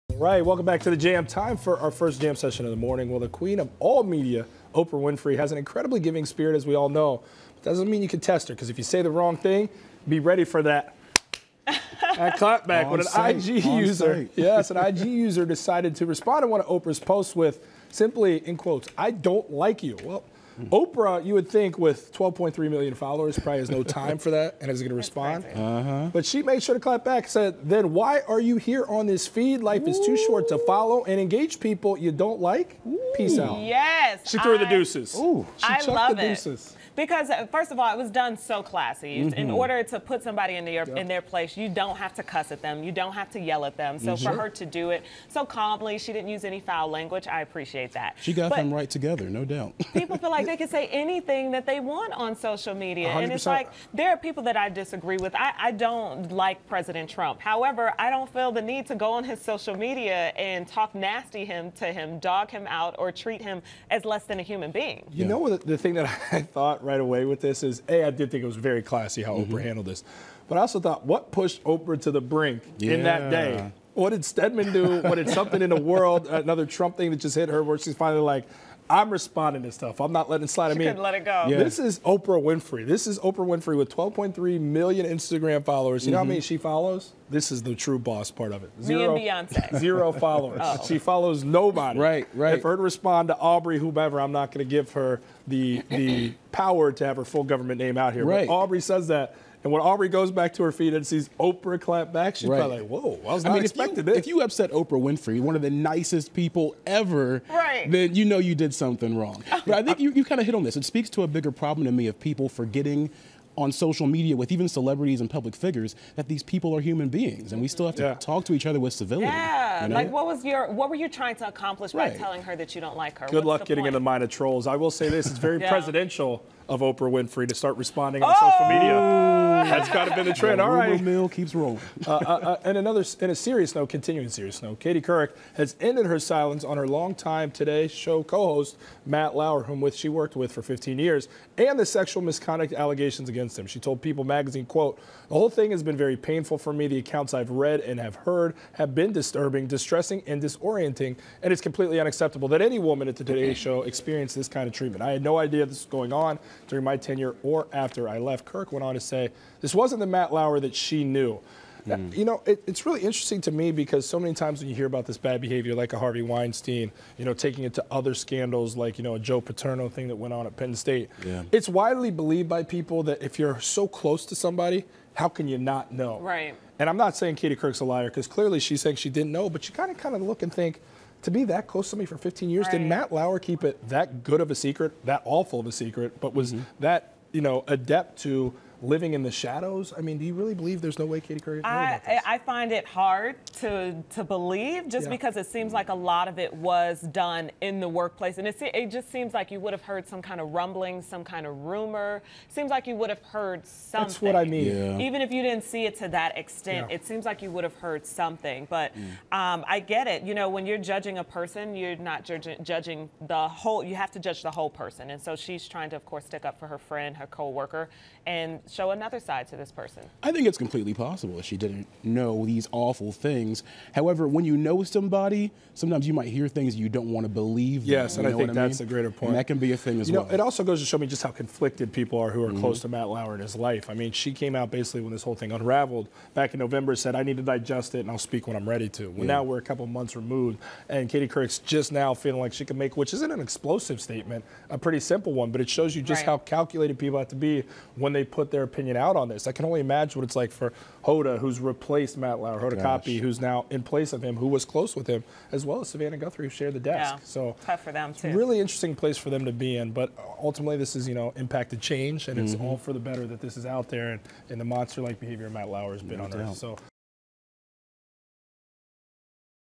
Oprah Silences Hater and Tells online Troll to get Lost
In this Jam session we chatted about Oprah clapping back on social media to an online troll.